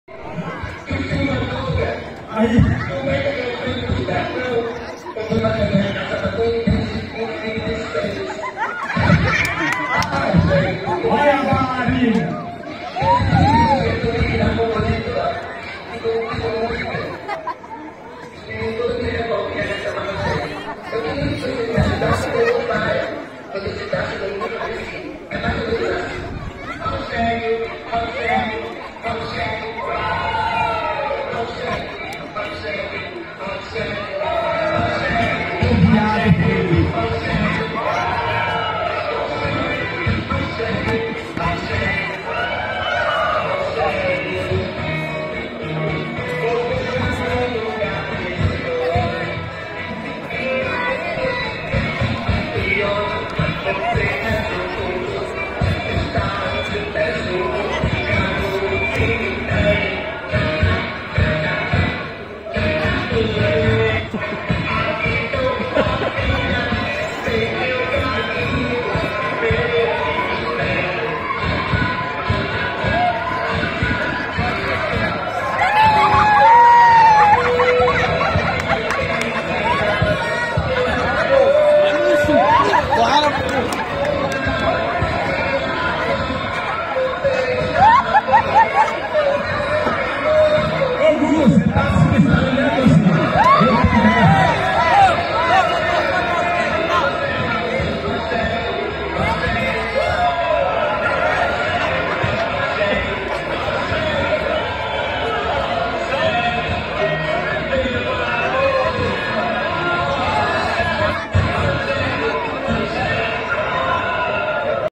Rogério Apareceu No Circo
E a animação foi grande quando Rogério apareceu no circo.